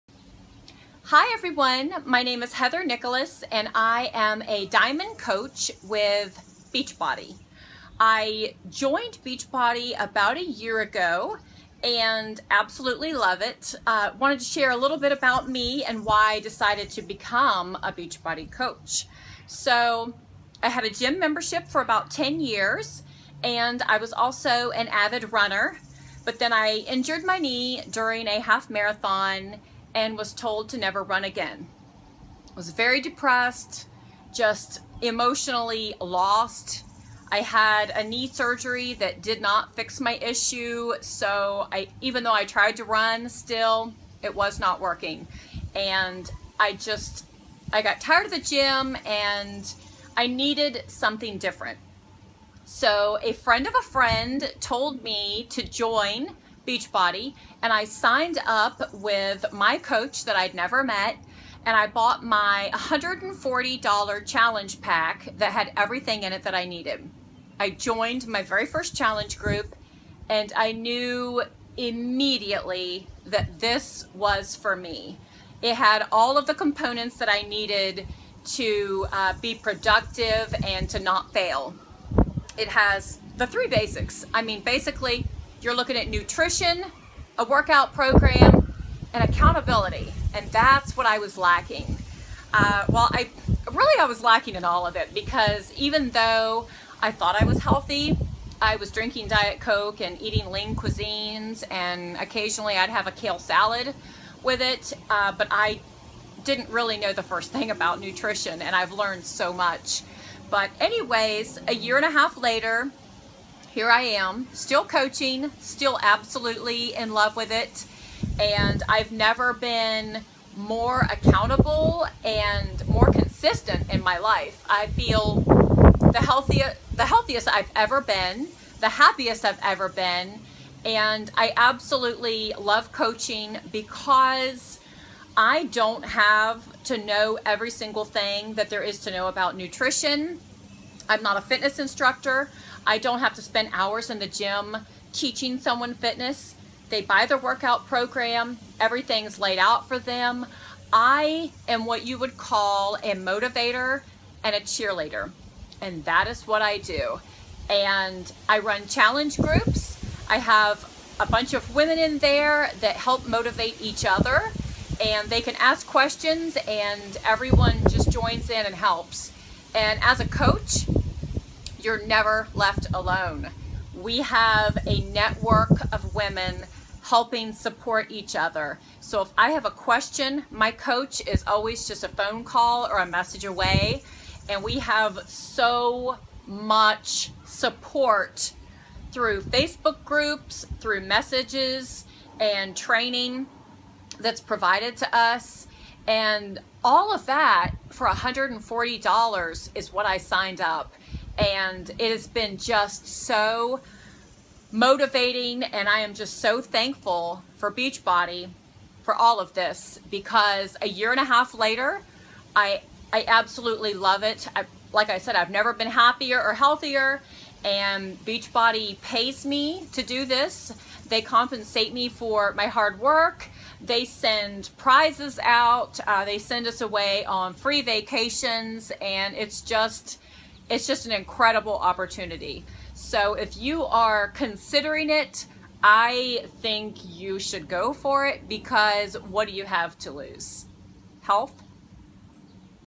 Why I became a Beachbody coach
Over a year later, a Diamond coach, I love what I do.